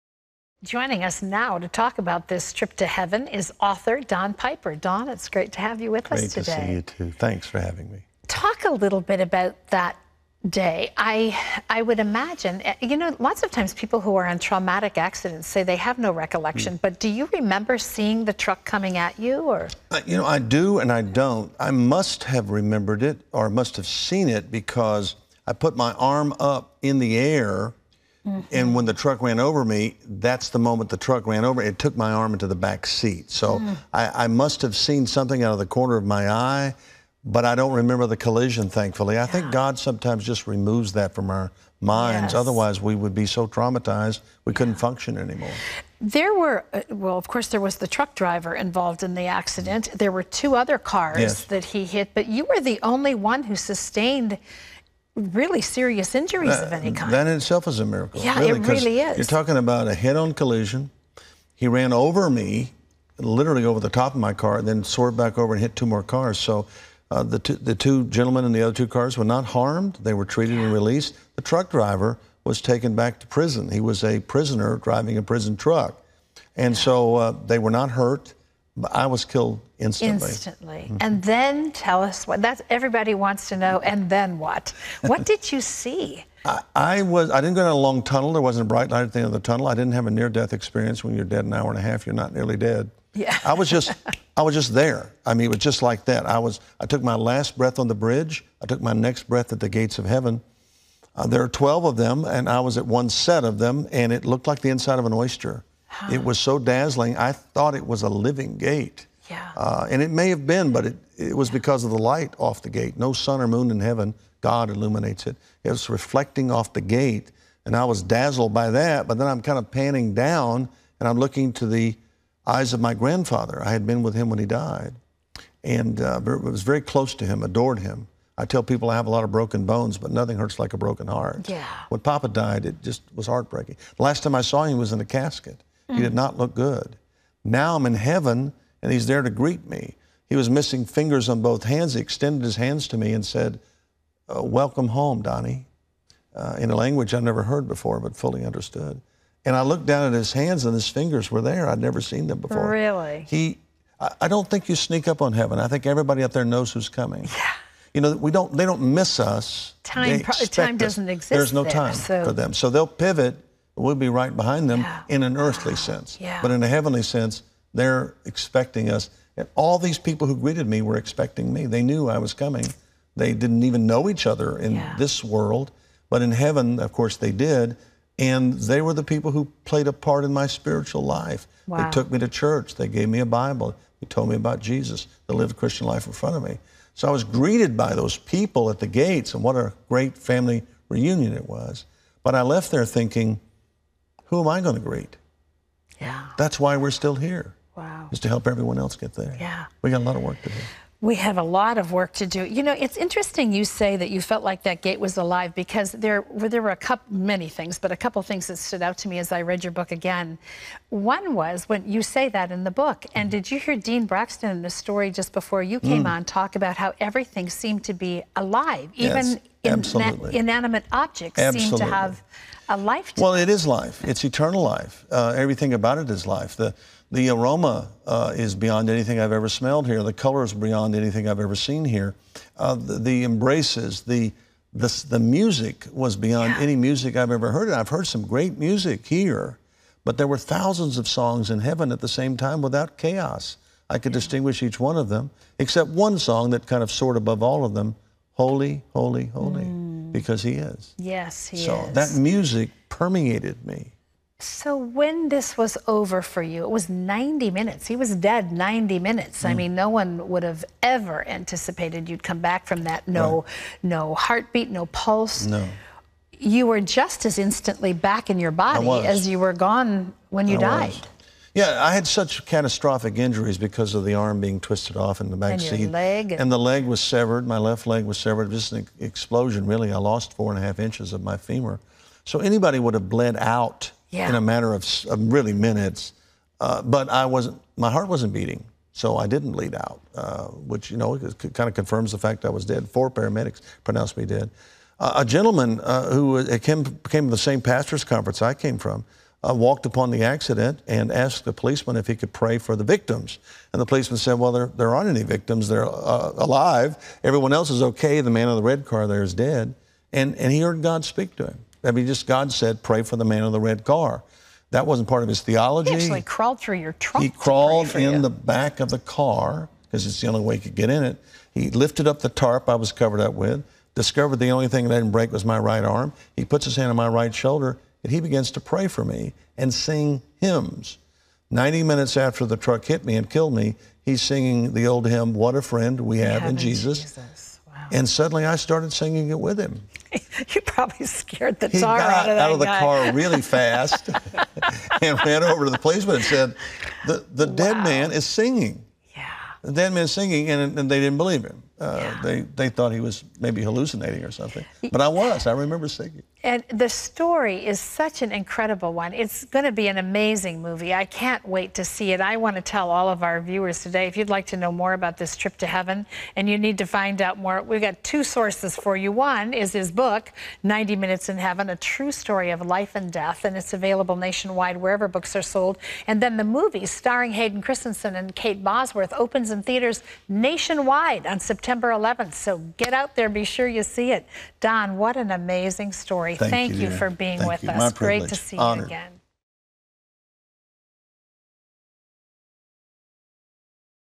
Don Piper Revisits Heavenly Experience
Author Don Piper discusses the upcoming movie adaption of Don’s best-selling book, 90 Minutes in Heaven.